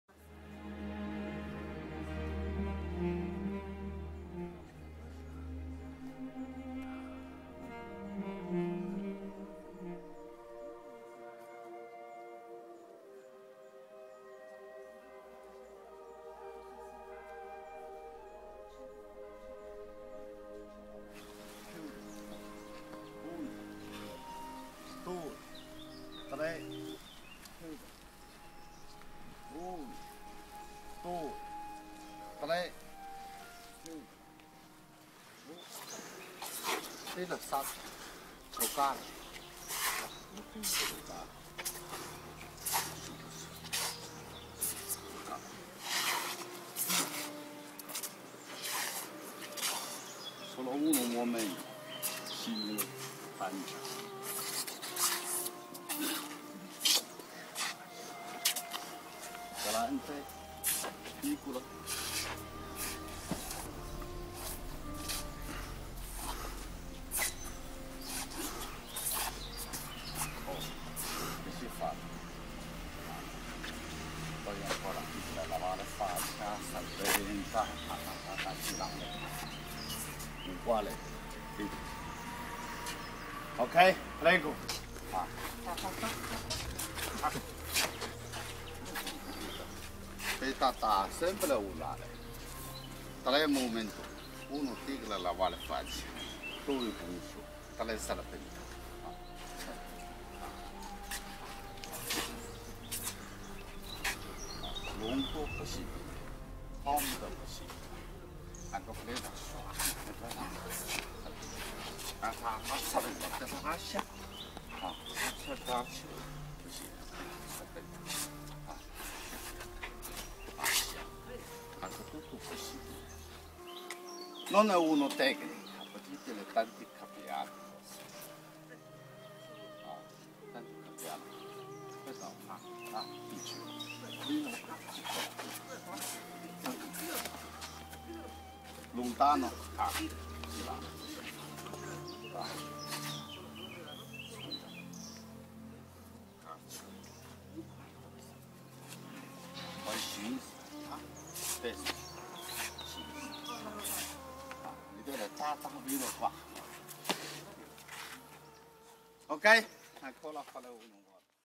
M° Yang Lin Sheng & M° Liu Chun Yan - Serpente Taiji Chen
Il M° Yang Lin Sheng e la M° Liu Chun Yan eseguono la tecnica del serpente del Taiji Chen; Seguono applicazioni